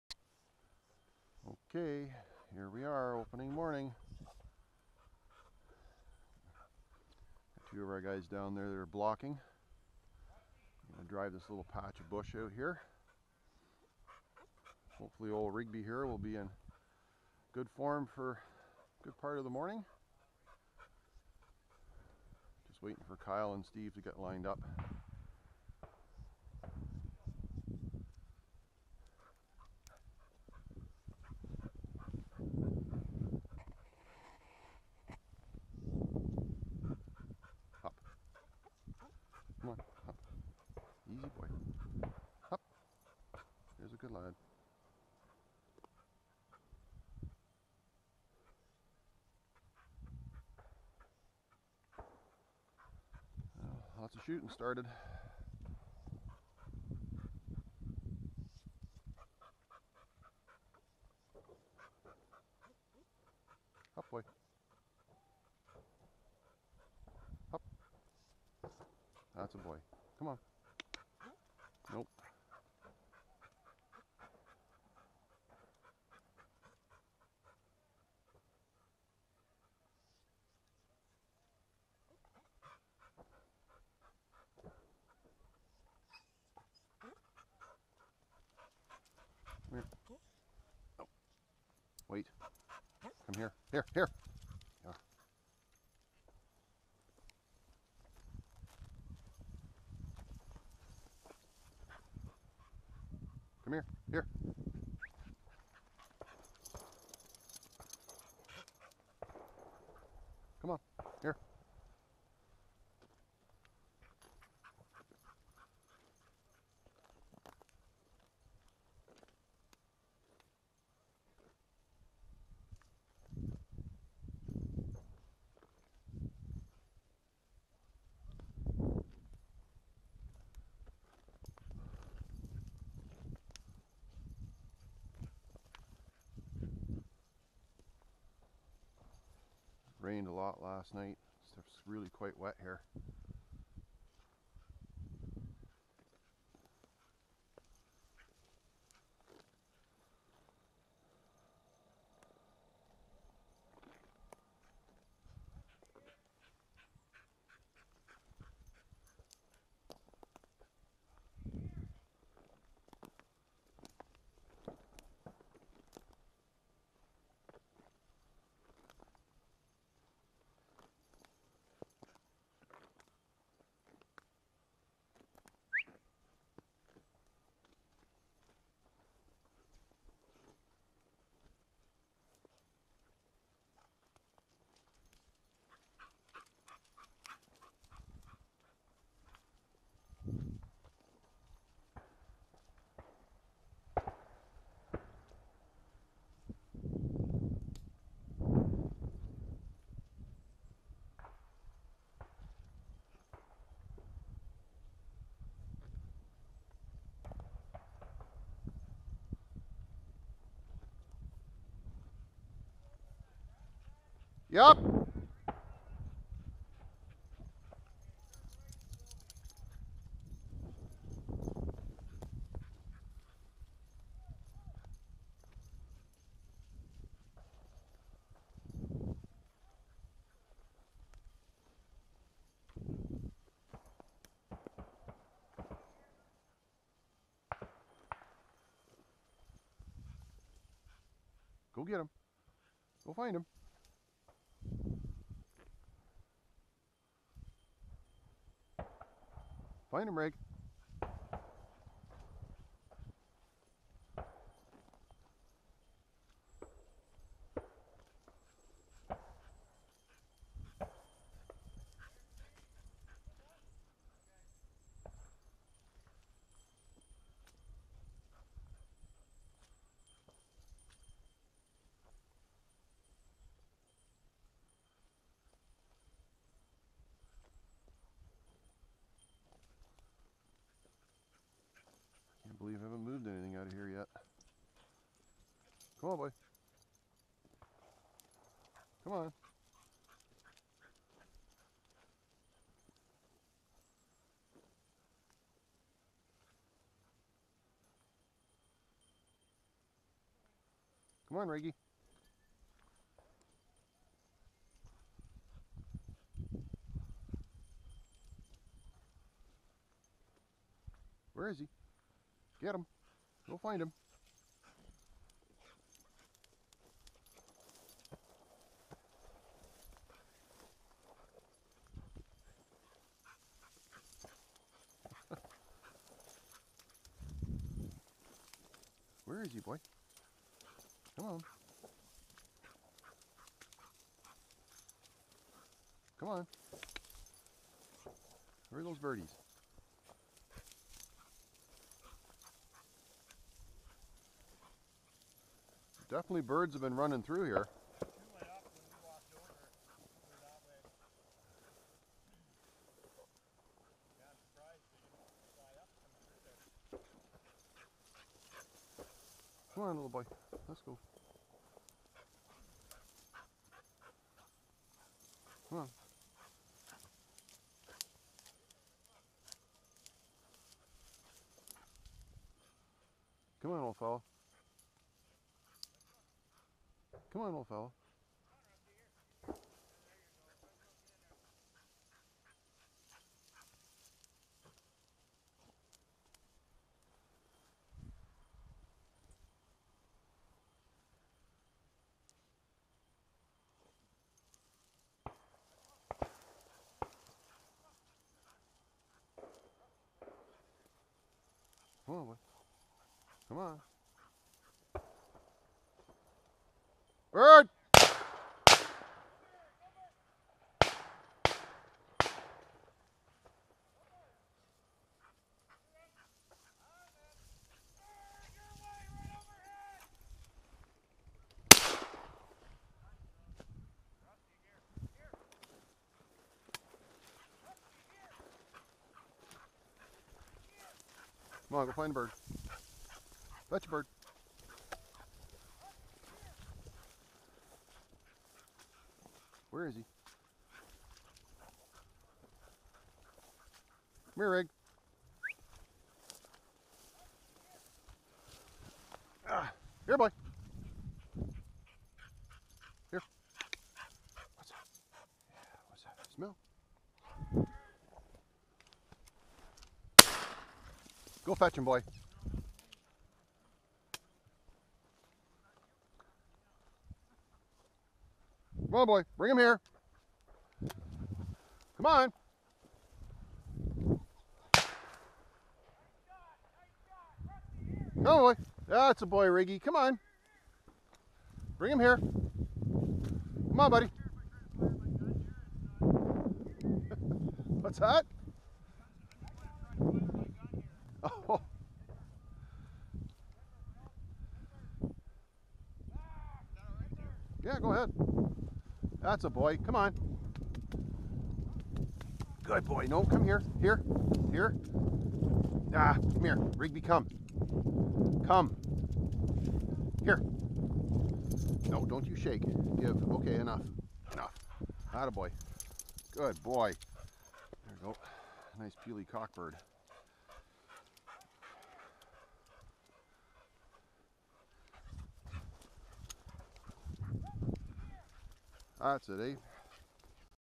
Cocker spaniel Rigbee hunting Pelee Island pheasants 2016
At 11 years of age in 2016, Rigbee is still hunting. A little slower and more deaf than usual, but still having fun.  A torn ligament in his left stifle in February 2017 has forced him to retire early.  In his retirement he's been helping out with training our young Welsh springer Bryn.  Thank you Old Buddy, for 11 memorable seasons.